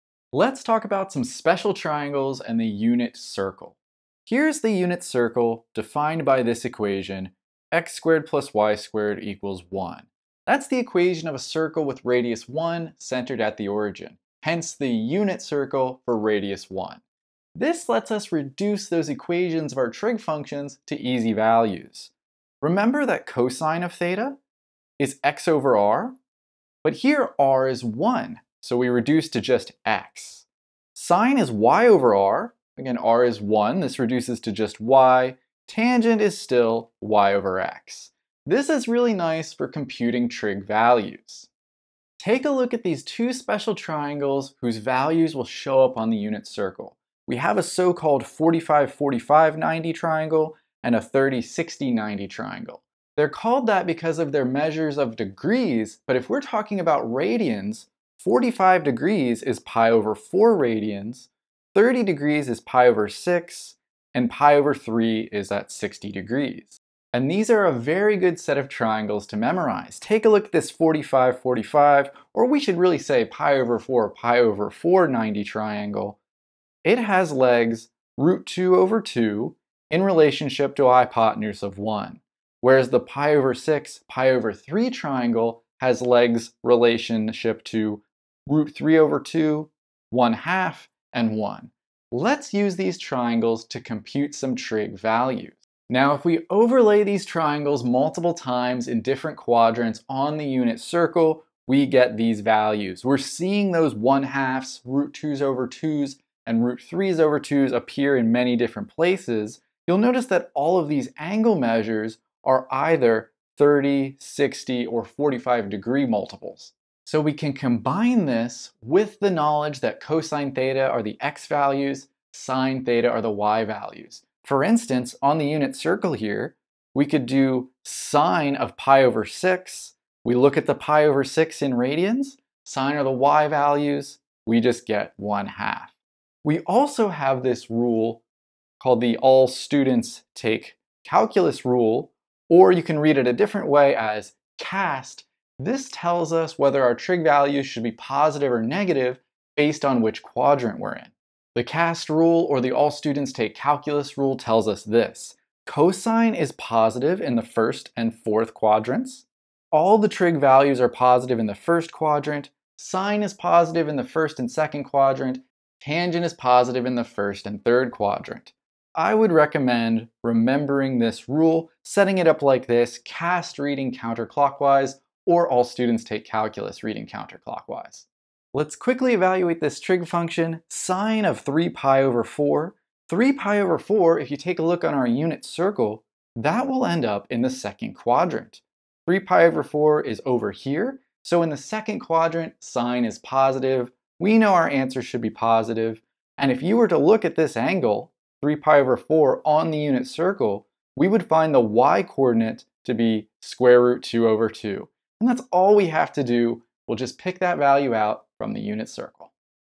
Unit Circle and Special Triangles (In Radians!) | Grade 12 Math Simplified | Wizeprep
Learn what a unit circle in radians is and how it help us with trig. We'll also show you another cool trick with special triangles (30 60 90 triangle and 45 45 90 triangle) in case you hate memorizing the unit circle.

In this video, you'll learn the answers to questions like:
 • How are special triangles related to the unit circle?
 • What are the special triangle rules?
 • What is the easiest way to memorize the unit circle?
 • What is the meaning of Soh CAH Toa?
 • What is the special triangle formula?

Want more unit circle and special triangle questions? You can also find a free printable unit circle